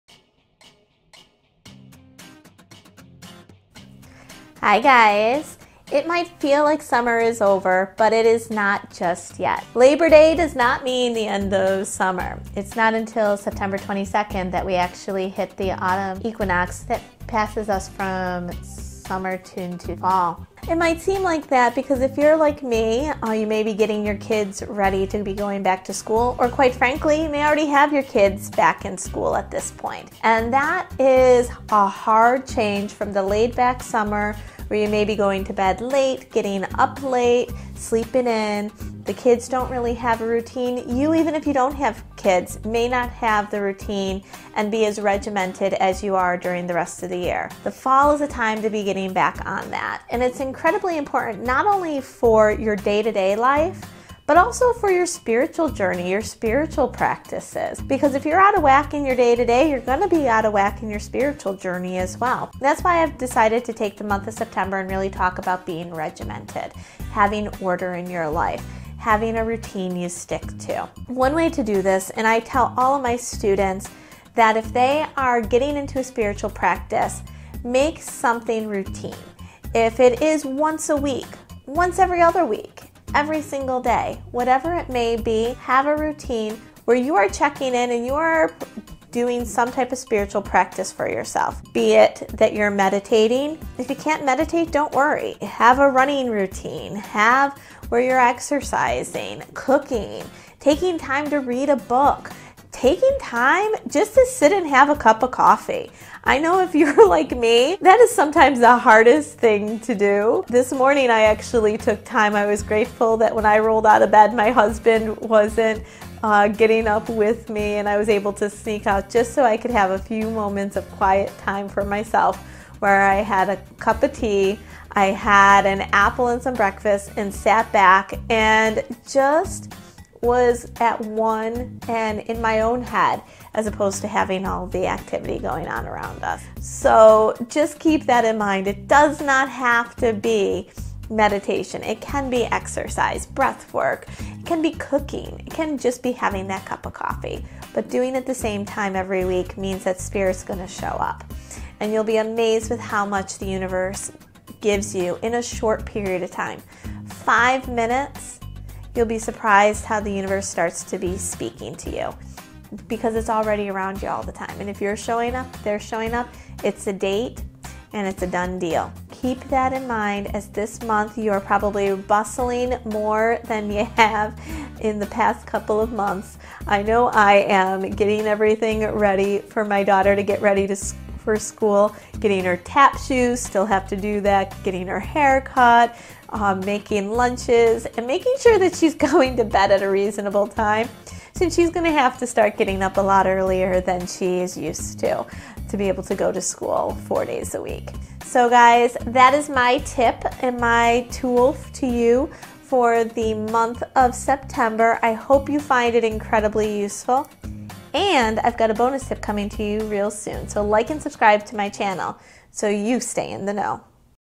Creating Order through regimen and routine
The month of September brings our kids back to school and signals the end of summer. As we get back to the grind, it can be helpful to bring a bit of routine to our lives. This is true for our spiritual practice as well.  Listen in as Dawn Lynn talks a bit about exactly that... And be sure to subscribe and follow her on social media for a bonus tip coming up in the next few days!

Dawn Lynn
Speaker, Author, Medium